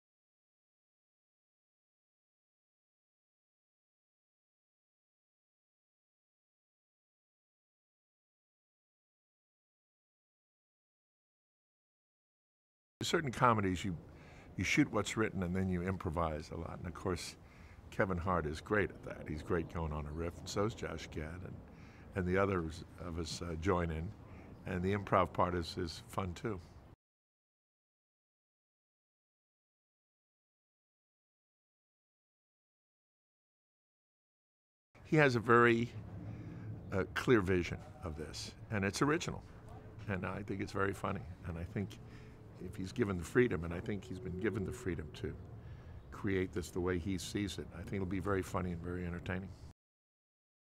The Wedding Ringer: Ken Howard "Ed" Behind the Scenes Movie Interview | ScreenSlam
A shy young groom needs to impress his in-laws, so he turns to a best-man-for-hire to help him out.

Director:
Jeremy Garelick
Writers:
Jeremy Garelick, Jay Lavender
Stars:
Kaley Cuoco-Sweeting, Alan Ritchson, Josh Gad